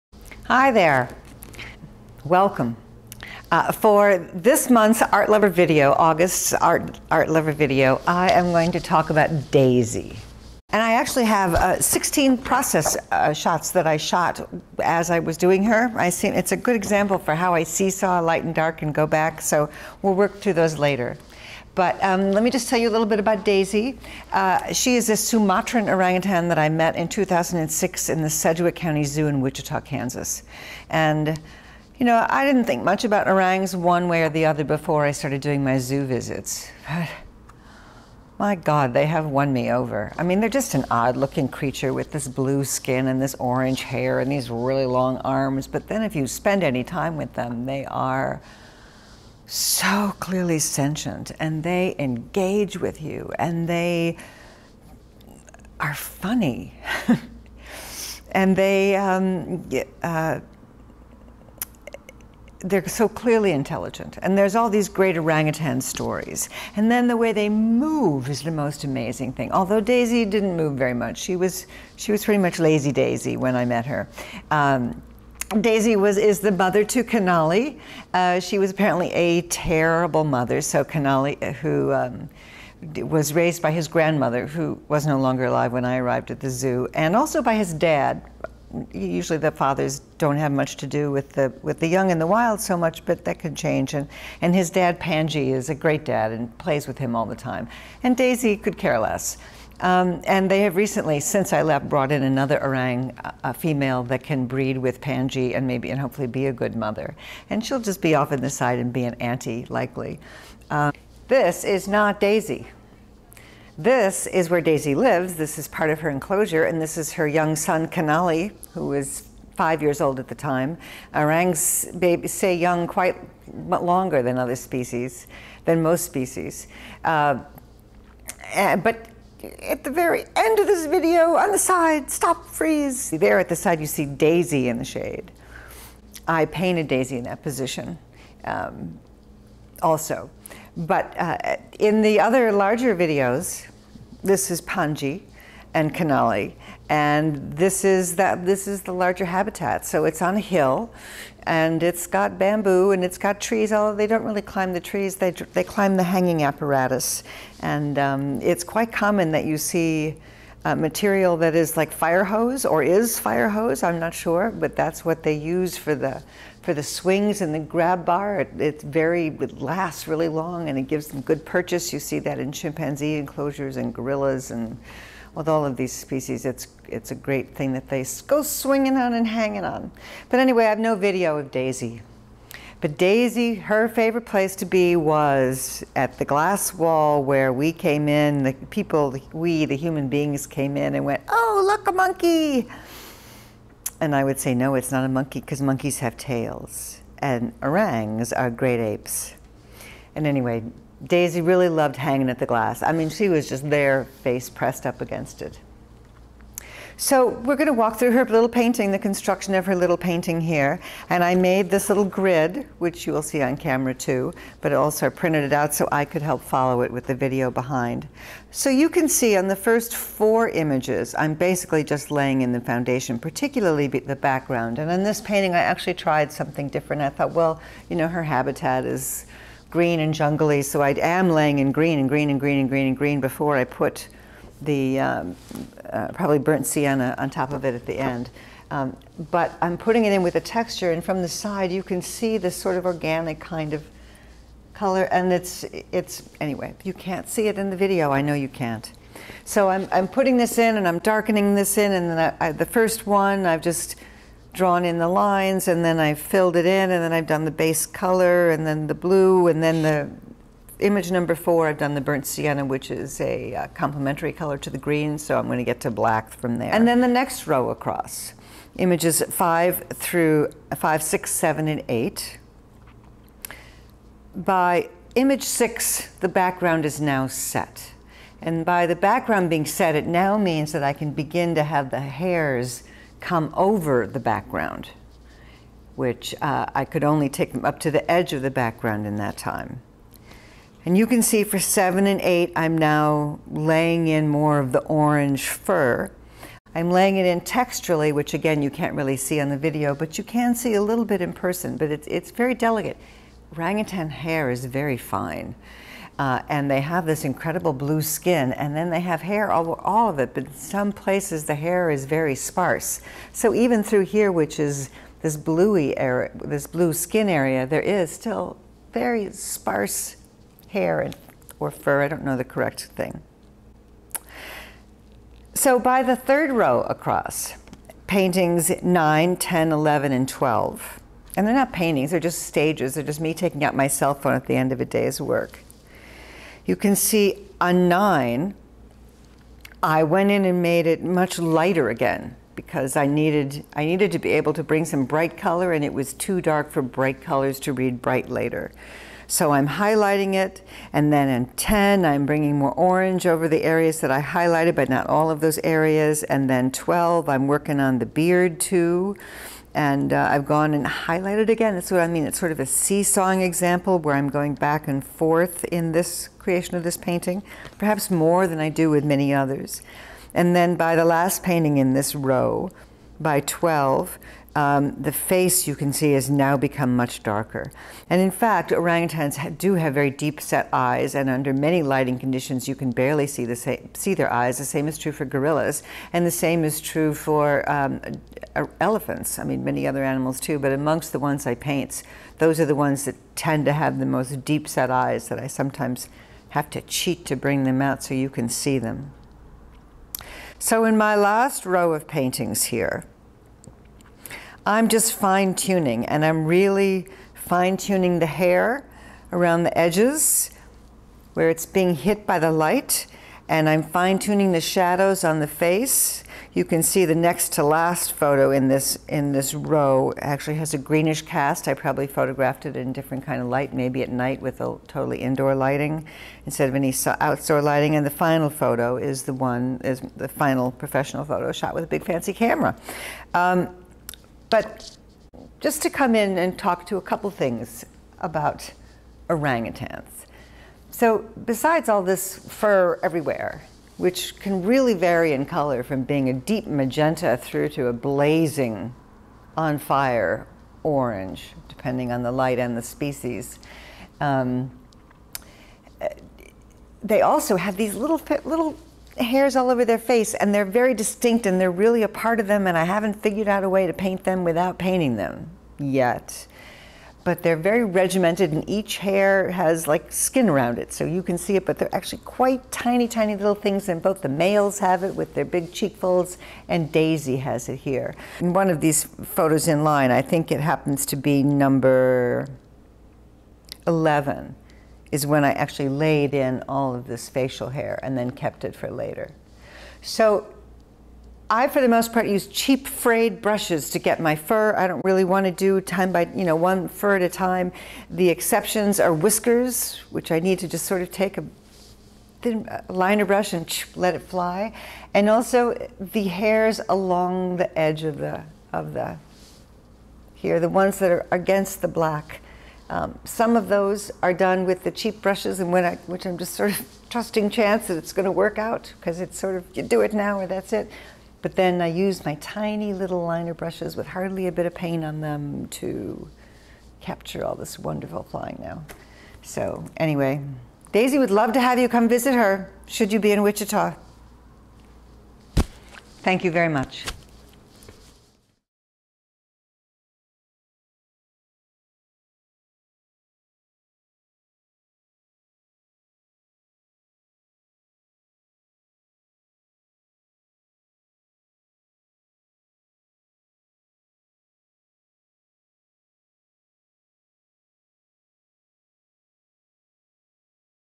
Art Lover: Daisy, a Sumatran Orangutan, 8/2018
This month's Art Lover video is about Daisy, a Sumatran orangutan I met at the Sedgwick County Zoo in Wichita, KS, and the oil portrait I made of her. I weave together 16 photos taken over the days I painted her portrait into a short tutorial guide into how I got from here to there. As an artist, an important foundation is to have a sense of the animal I am painting. Daisy is curious and engaging and likely a great observer of human behavior.